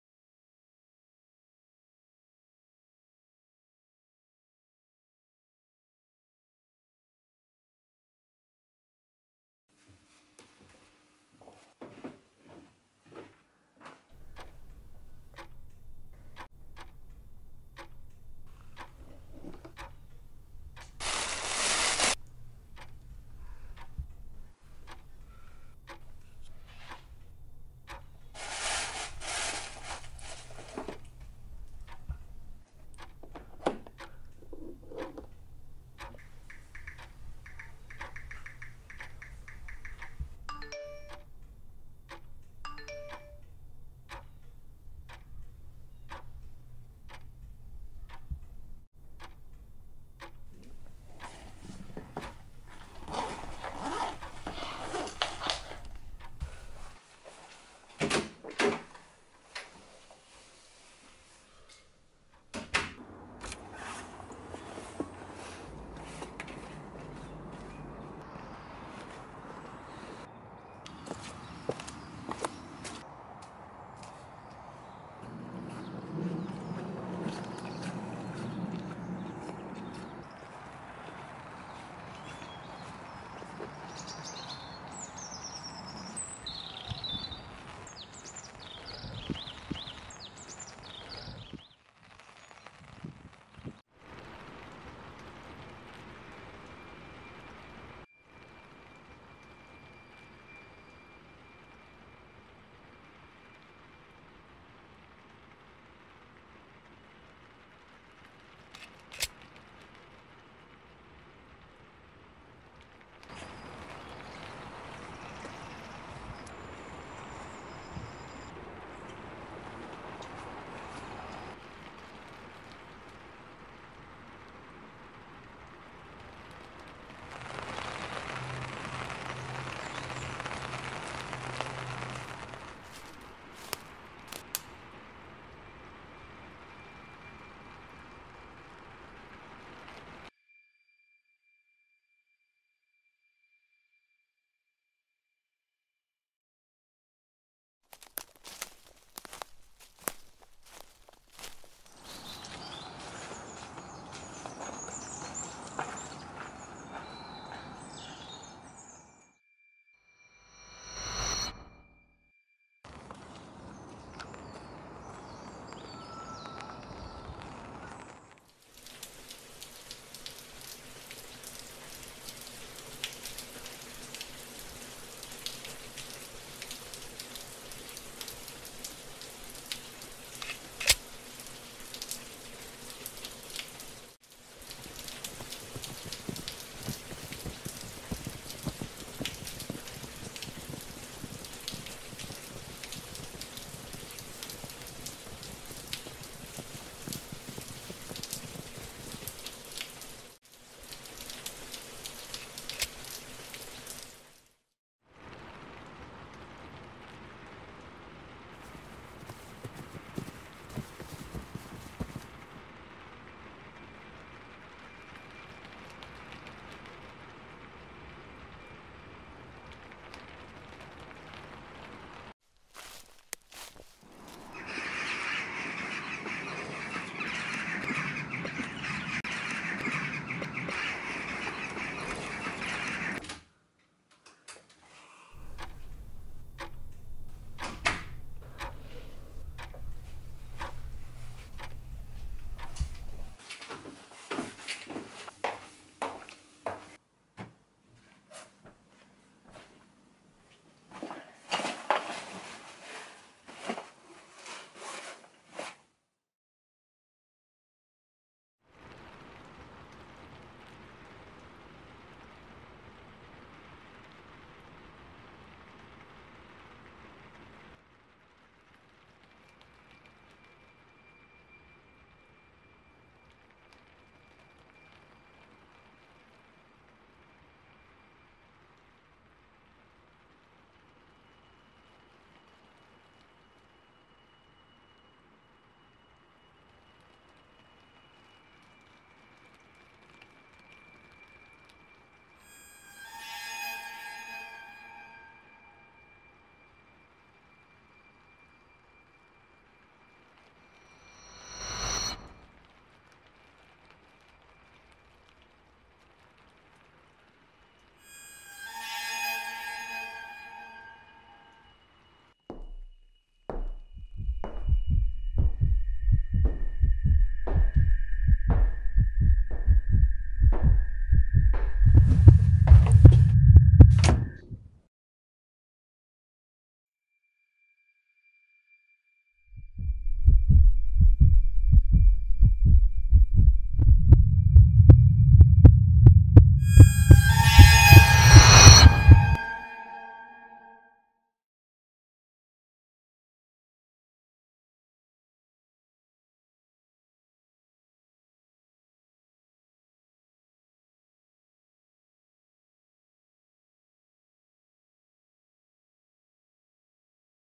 The Dark Side - Stalker (short film)
My first attempt in making a short film with the dark side as theme for BTEC Unit 10 (Film Production)